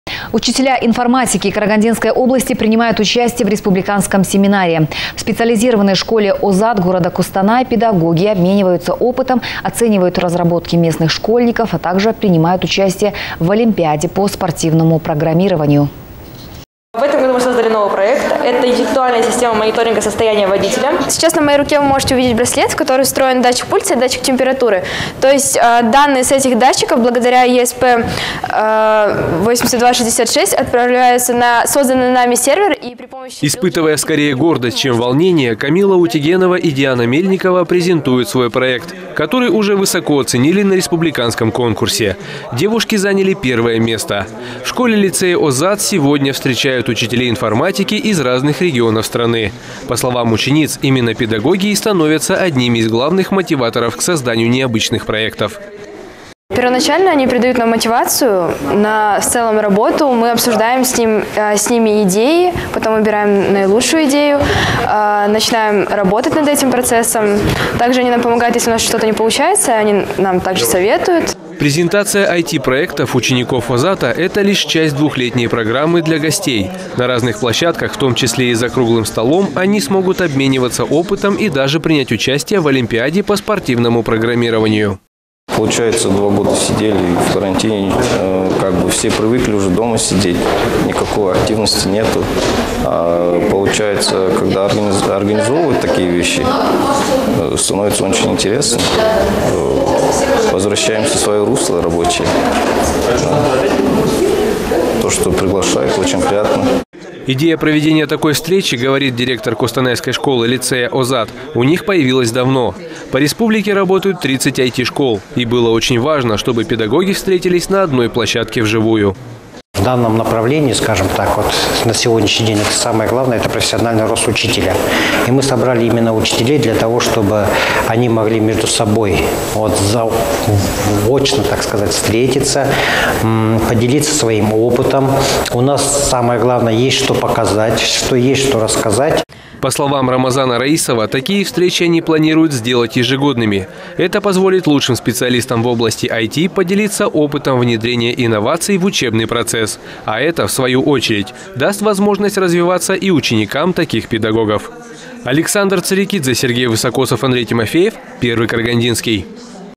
Камила Утегенова и Диана Мельникова с проектом, который оценили на республиканском конкурсе.
Презентация IT-проектов учеников «Озата»